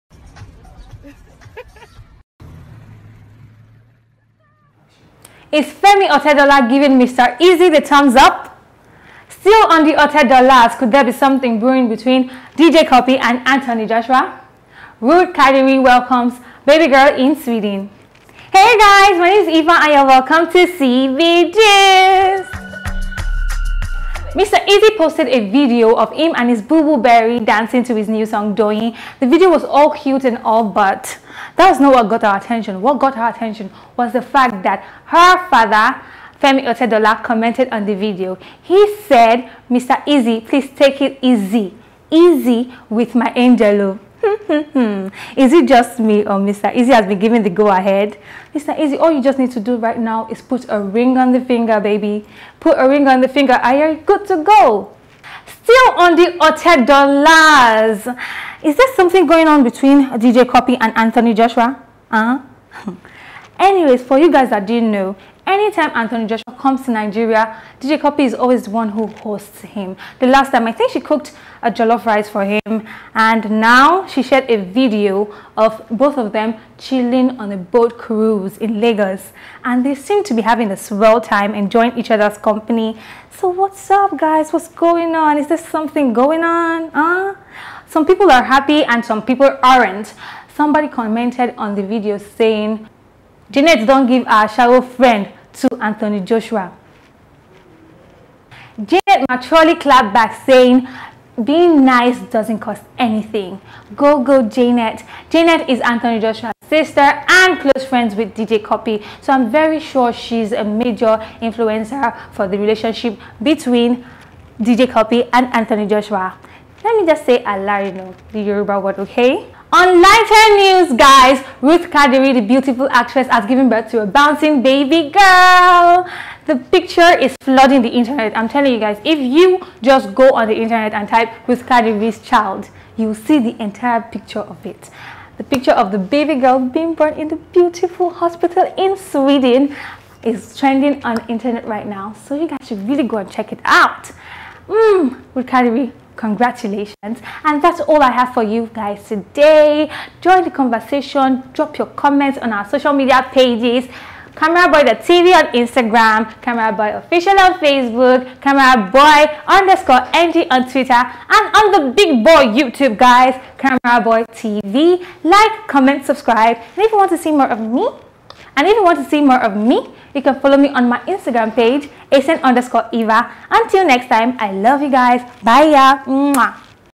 What's more to MrEazi eaziness with TeniOtedola
On CBGist today with eva, whats there more to MrEazi eaziness with TemiOtedola? Is there something ongoing with DJCUPPY & AnthonyJoshua? and Congrats are definitely in check for RuthKadiri as she welcomes a baby girl...

Video Edited by @Kolaabass
Produced and Directed by Martinsbassey
DOP - @winnerkiss